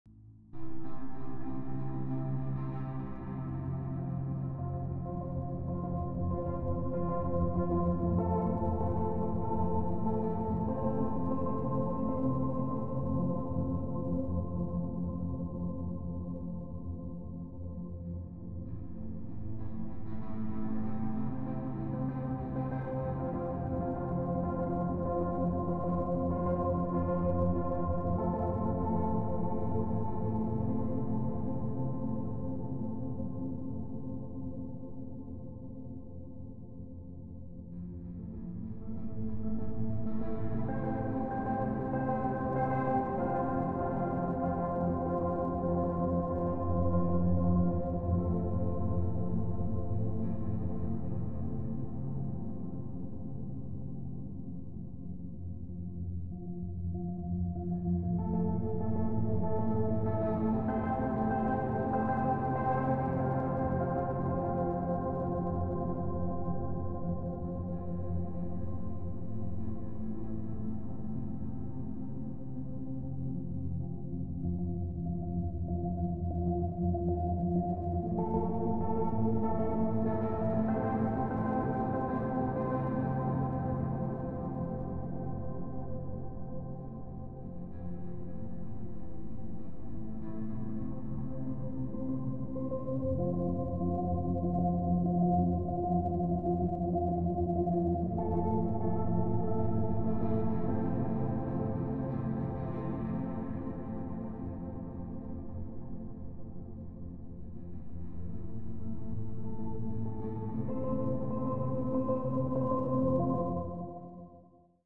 Liverpool Sunrise from the Wirral | DJI Mini 3 Pro | 4K
Drone video of the sunrise over Liverpool from Wallasey beach